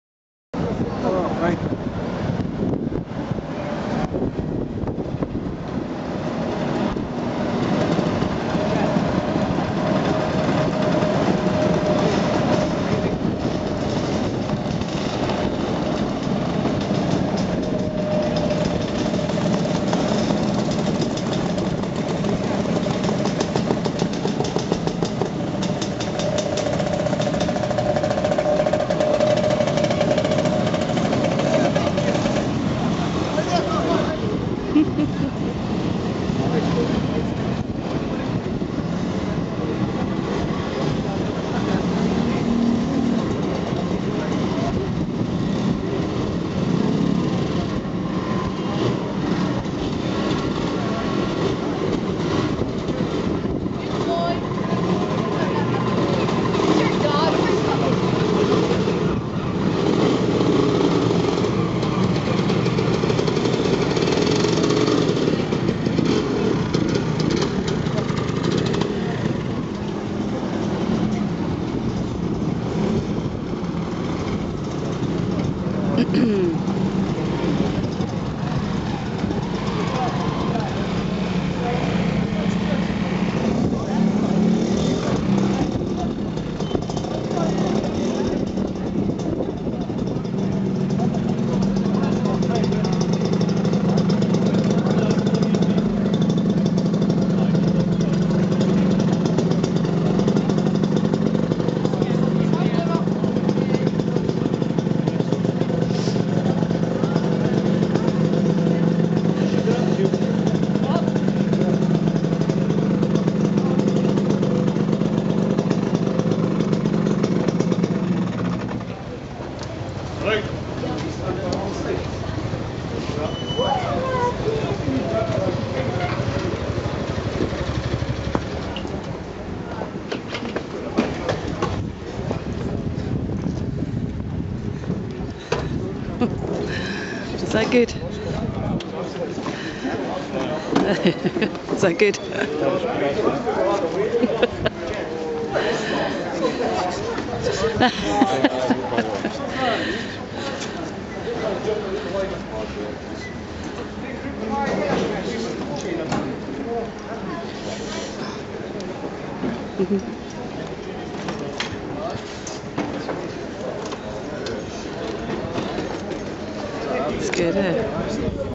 march of the mods 2014 aberdeen
scooters arriving for the quadrophenia film in aberdeen